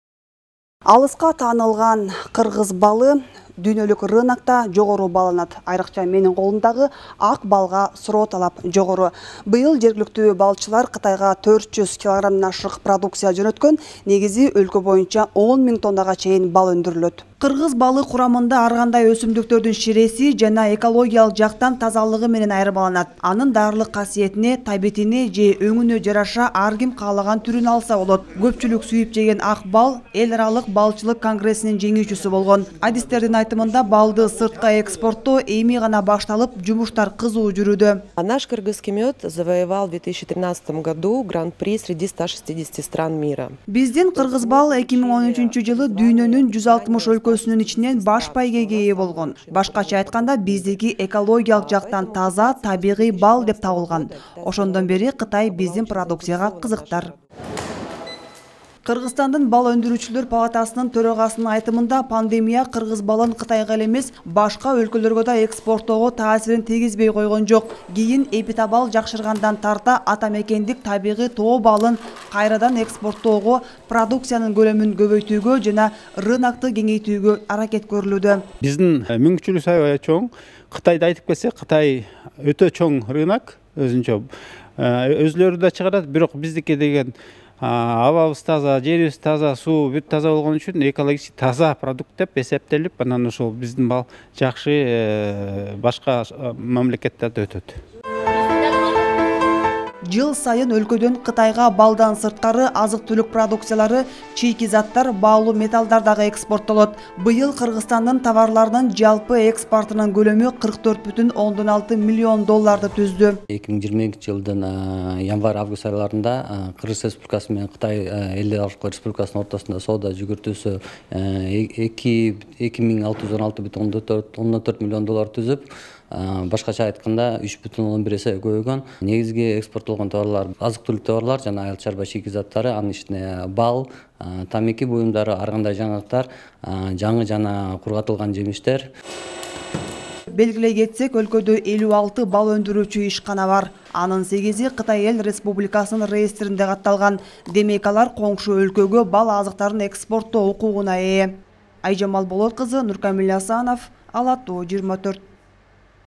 Кытай менен тышкы сооданын көлөмү өстү
Быйыл Кыргызстан менен Кытайдын тышкы соода жүгүртүүсү 2 миллиард 616 миллион долларды түздү. Бул өткөн жылга салыштырмалуу 3,1 эсеге көп. Маселен, товарлардын жалпы экспортунун көлөмү 44,6 миллион каражатты түзөт. Учурда экспортко баалуу металлдар, чийки заттар, азык-түлүк товарларынан сырткары, экологиялык жактан таза кыргыз балы да жөнөтүлүүдө.